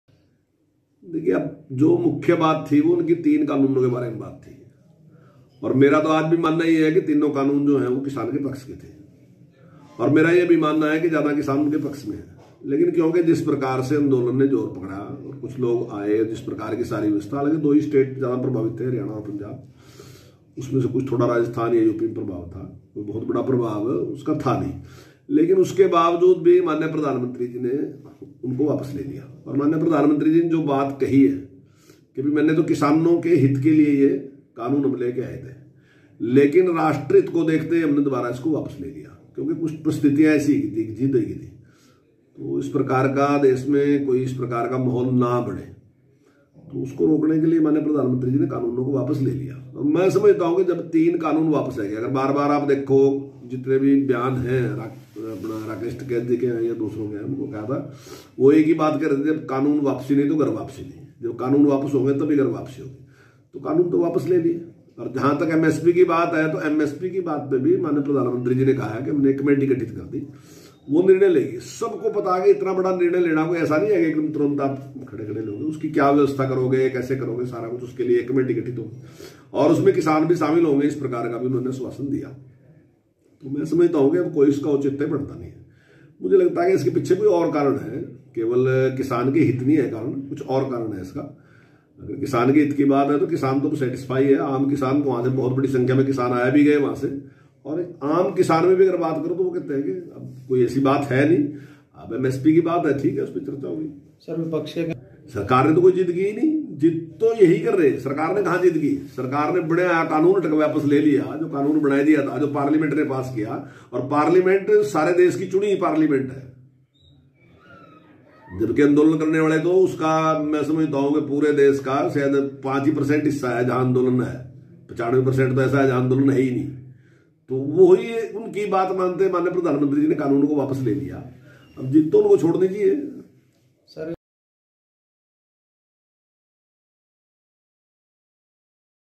किसान आंदोलन पर कंवरपाल गुर्जर का बड़ा बयान | Kanwarpal Gurjar big statement on the farmers movement
हरियाणा के शिक्षा मंत्री कंवरपाल गुर्जर (Kanwarpal Gurjar) ने किसान आंदोलन को लेकर कहा कि तीनों कृषि कानून वापस (farm laws repeal) ले लिए गए हैं. अब किसानों को जिद छोड़ देनी चाहिए. किसान नेताओं का कहना था कि कानून वापसी तो घर वापसी, तो अब वहां बैठने का औचित्य ही नहीं बनता. मुझे लगता है इसके पीछे कोई और कारण है.